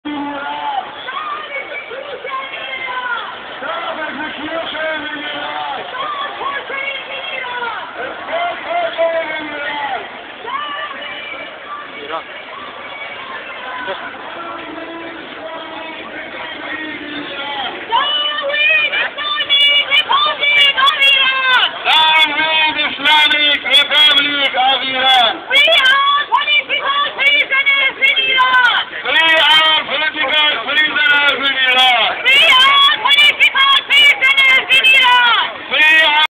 Egyptian Protesting in Dundas & Yonge
Calling for the removal of Egyptian President Hosni Mubarak.
(filmed on 2011/01/29 by my bro Vicente Chang)